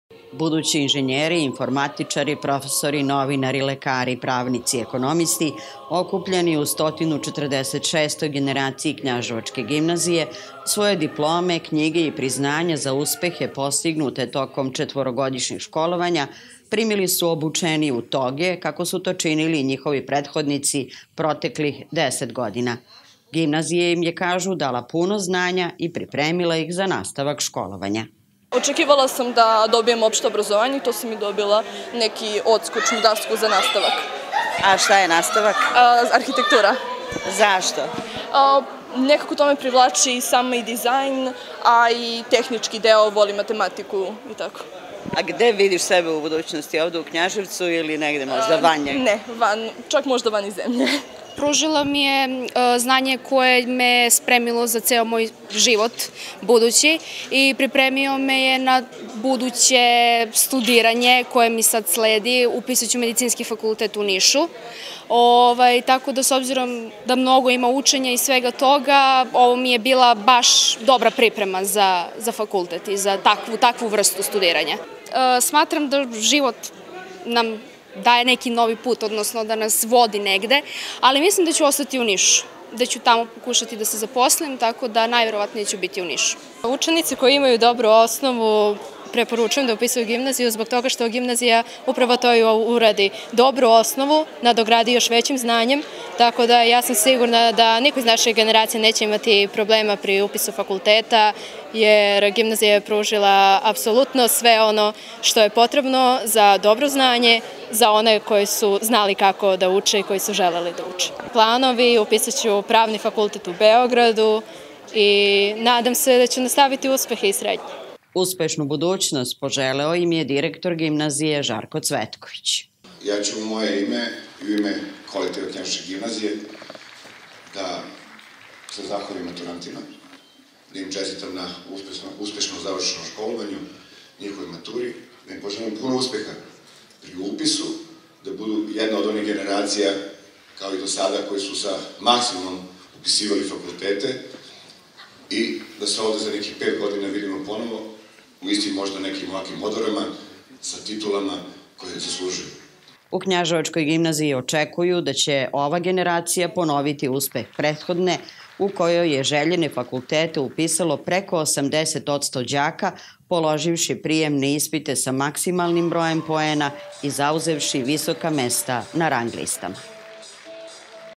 Ispraćena 146. generacija gimnazijalaca
Knjaževačka gimnazija organizovala je sinoć u Domu kulture svečanost na kojoj su 146. generaciji maturanata uručene diplome i priznanja. Učenicom generacije proglašena je Ivana Vušković, učenica IV/1 prirodno – matematičkog smera.